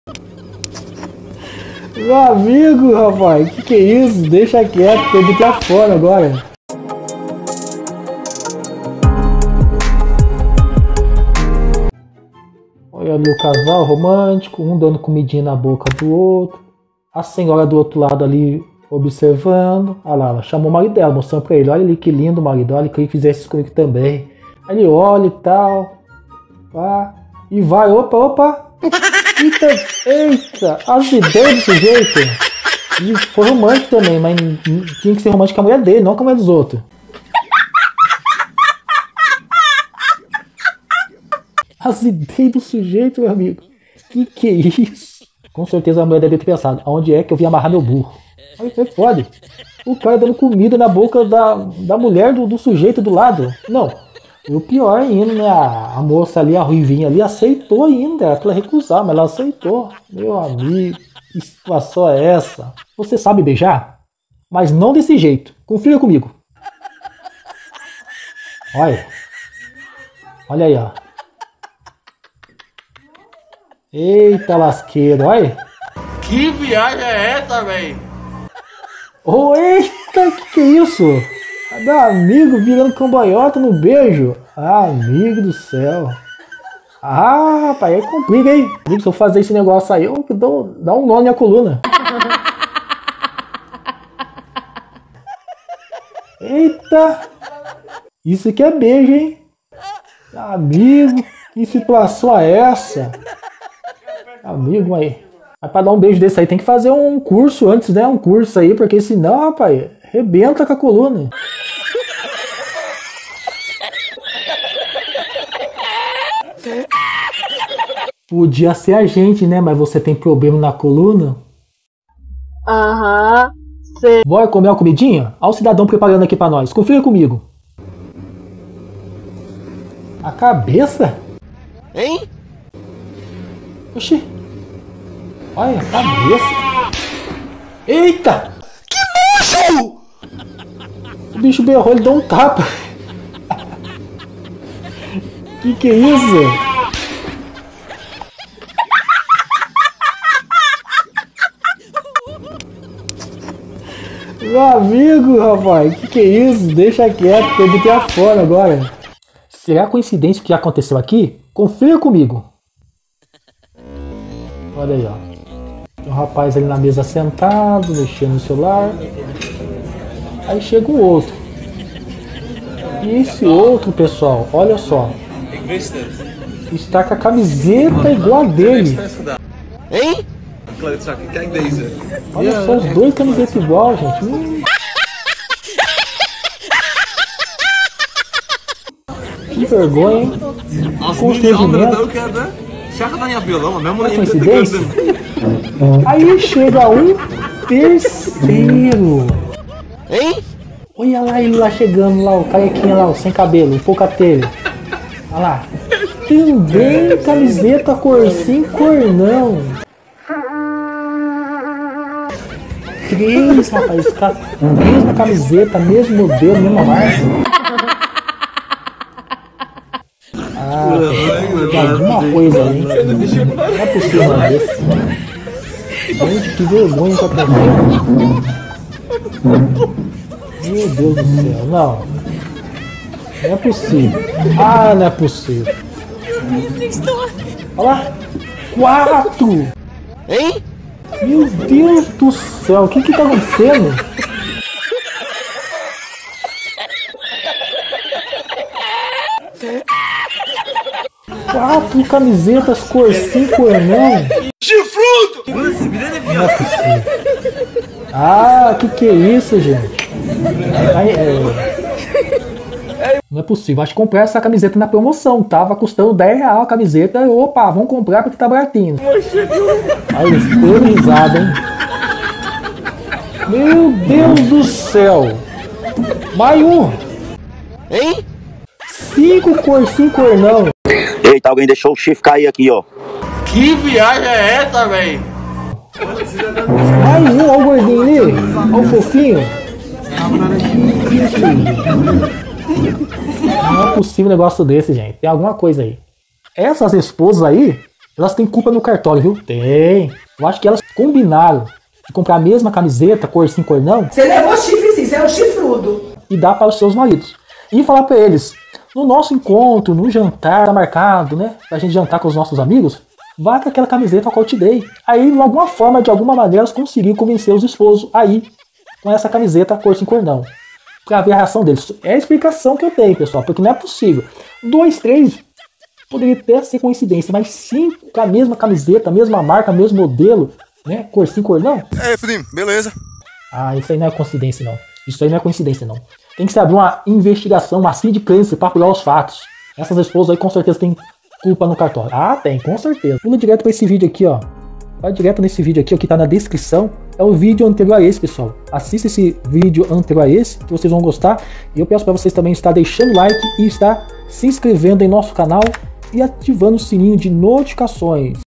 React Vídeos Comédias
EITA LASQUEIRA A CABEÇA DE BODE BERRO😱😂. Neste vídeo eu faço um react de vídeos engraçados. Nele estou reagindo a vídeos de tirar o fôlego de tanto rir.

0:00 Introdução
0:07 Vinheta
0:13 Na frente do marido dela
1:04 Beijo diferente
2:06 Comida que berra?
2:42 Elas fizeram isso com eles
5:43 Vídeo anterior